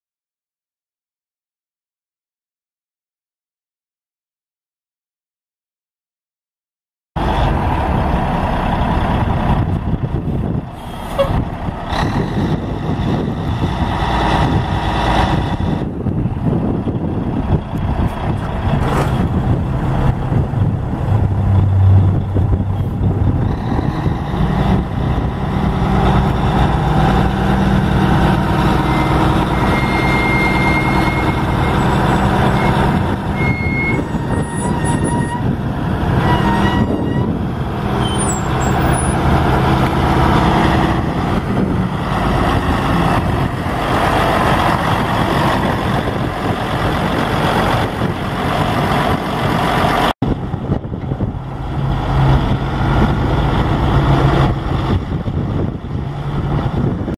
1985 Caterpillar 980C wheel loader for sale | sold at auction April 24, 2014
1985 Caterpillar 980C wheel loader
25,324 hours on meter
Caterpillar six cylinder diesel engine
Four speed shuttleshift transmission
Enclosed cab
Heat
Tilt
Two stick loader control
130"W loader bucket
Smooth bolt-on cutting edge
Hole in bottom of bucket
Work lights
Rear counter weight
26.5-R25 tires
60,755 lbs. base operating weight
Lower glass on left hand door missing
Windshield cracked
Serial 63X06275
Unit# 5404